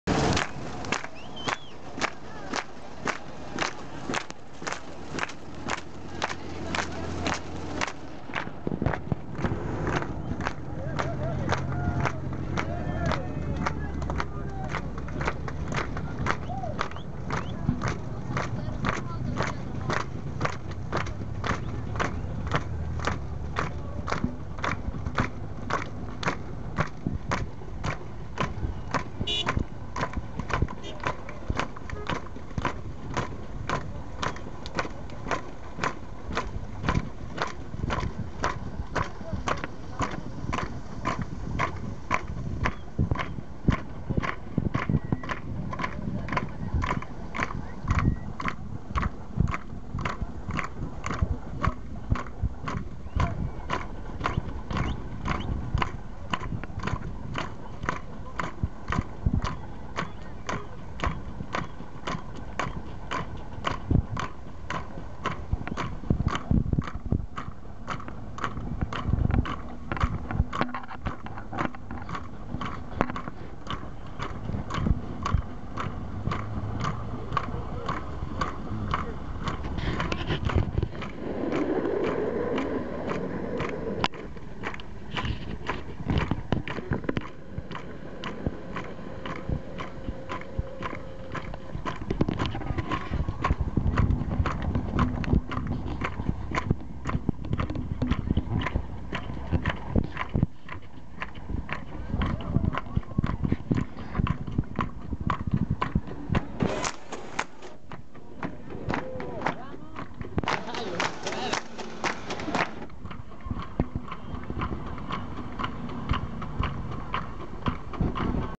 Chos Malal - Mineros en la Ruta 40
Mineros de Andacollo Gold reclamando en Chos Malal por sueldos adeudados y continuidad laboral.
Abril 2015